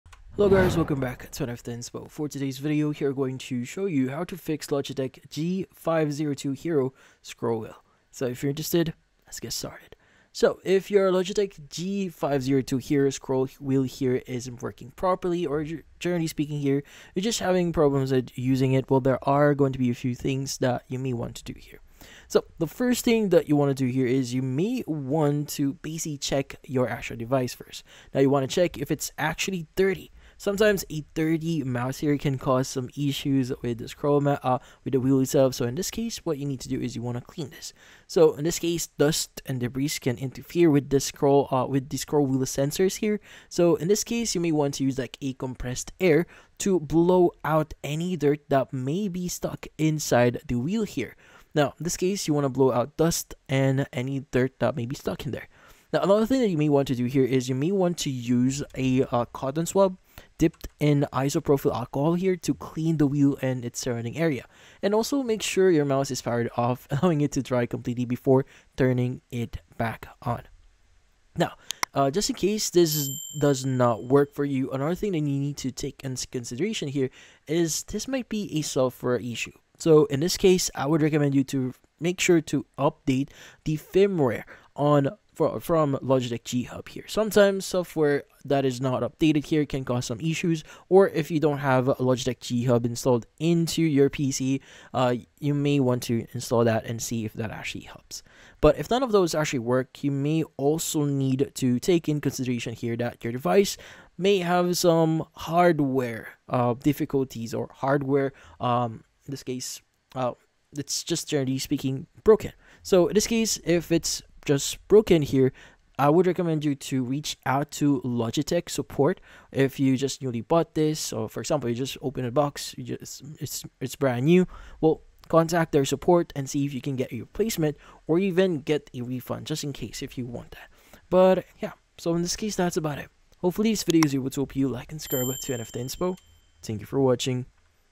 How To FIX Logitech G502 Hero Scroll Wheel
If your Logitech G502 Hero scroll wheel is not working properly, then this troubleshooting guide will be perfect for you!